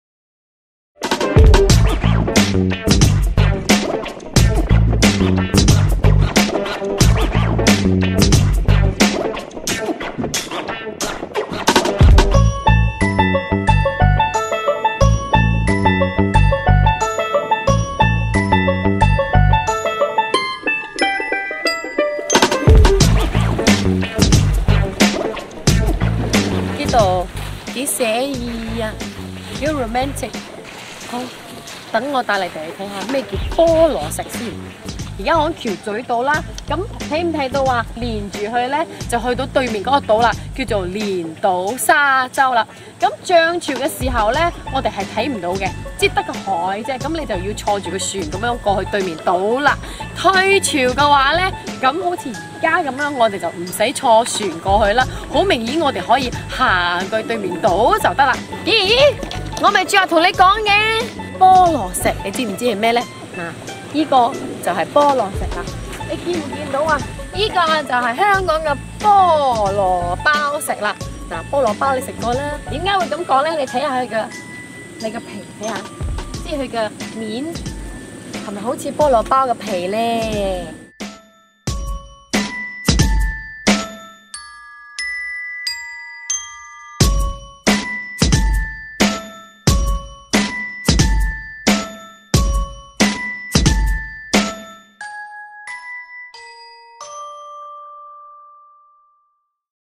香港 西贡桥嘴洲 菠萝包石 SAI KUNG Sharp Island Hong Kong
風化作用使岩石外層的礦物膨脹或 收 縮 , 最 後 引致岩石外層像洋蔥般層層剝落。頁狀剝落使石英二長岩的外貌有如「菠蘿包」。
Weathering causes minerals in the outer layers of the rocks to expandor contract, results in the outer layer of the rocks peeling off like thelayers of an onion. The quartz monzonite resembles the surface of a“pineapple bun” due to exfoliation.